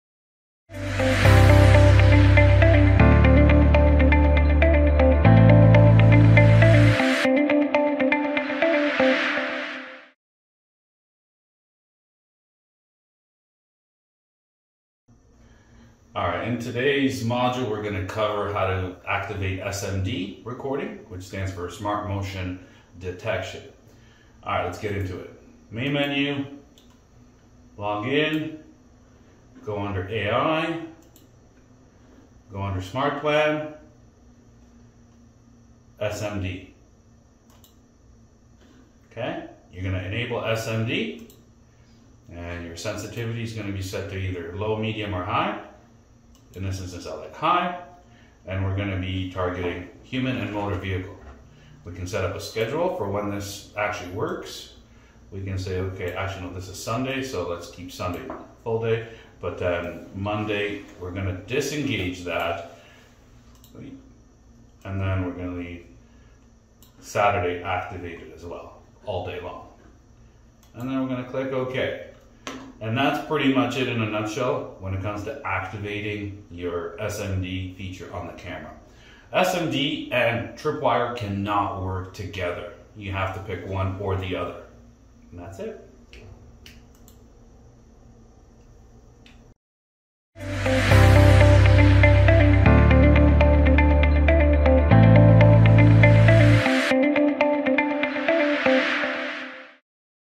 Dahua Smart Motion Detection (SMD) Setup
In this quick tutorial, we walk you through how to set up the Dahua Smart Motion Detection